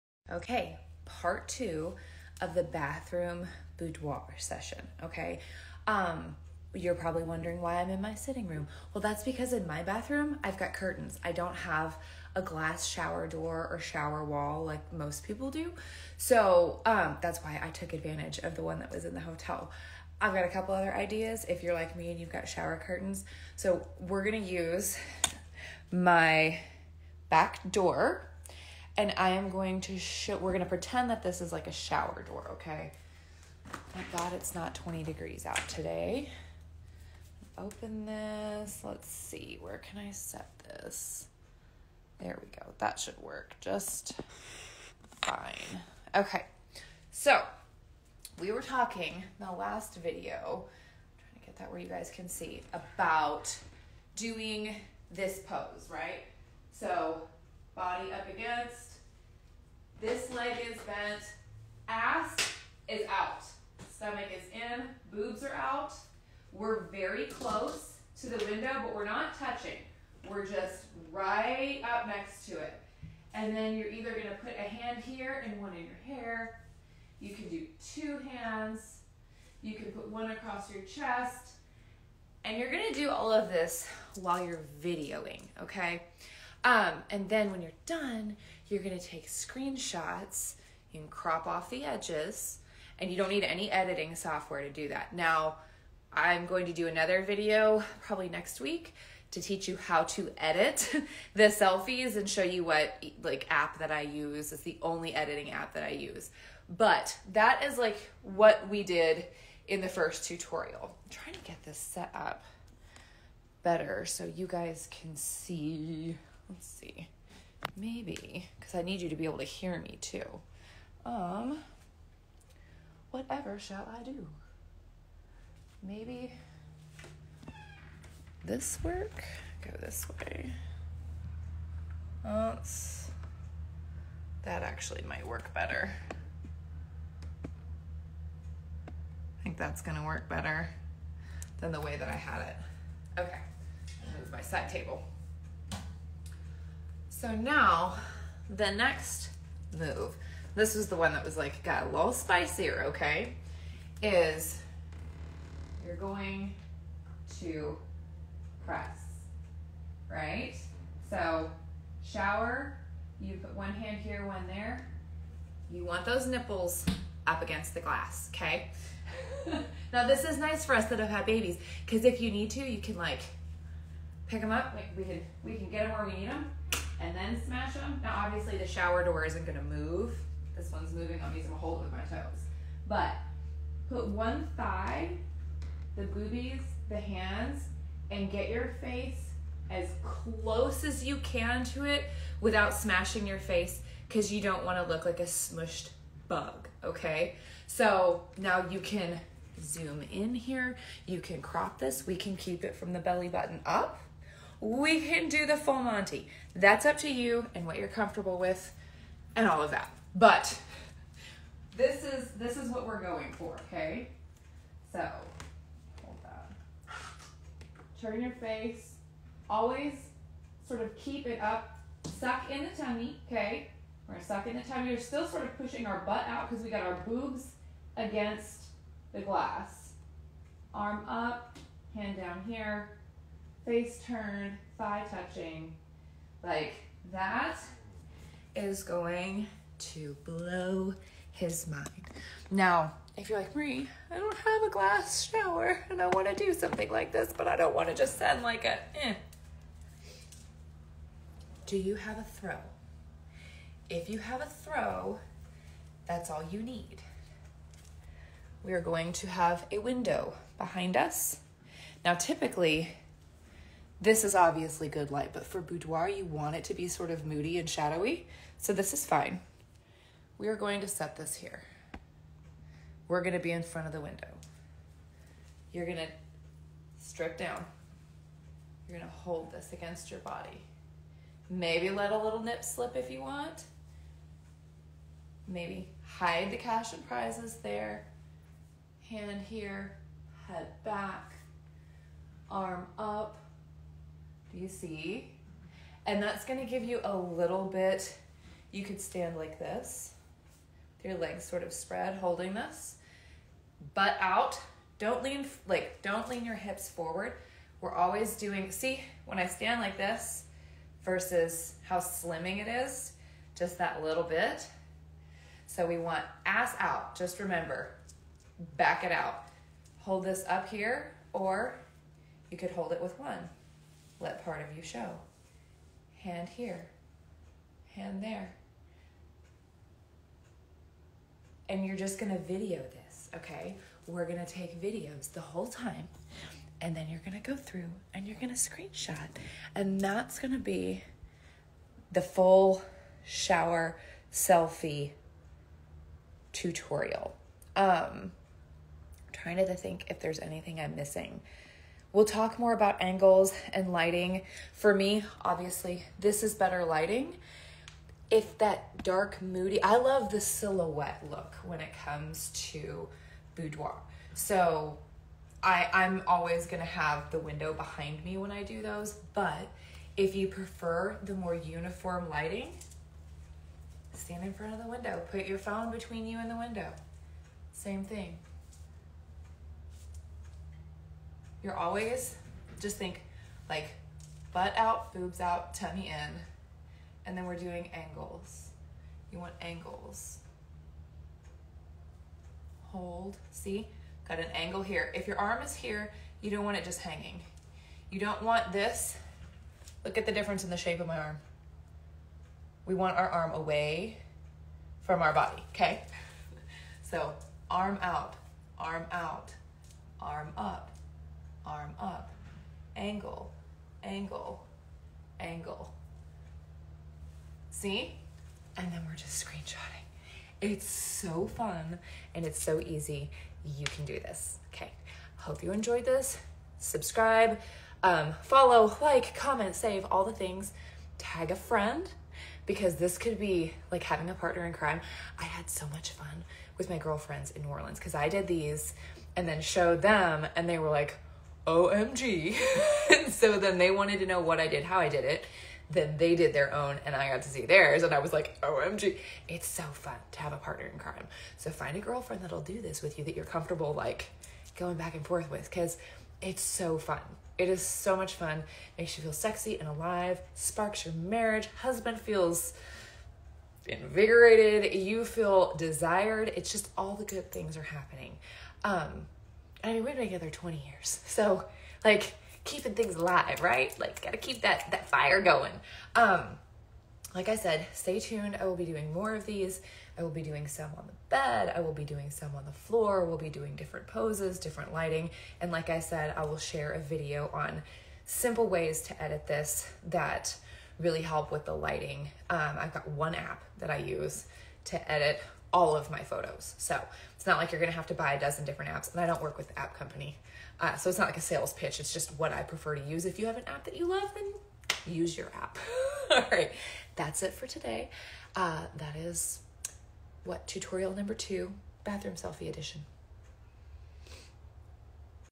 Shower Selfies part 2 | DIY Boudoir Photos
Here's how to take your own boudoir photos. Next week's tutorial will be HOW TO EDIT these smokin’ hot pix!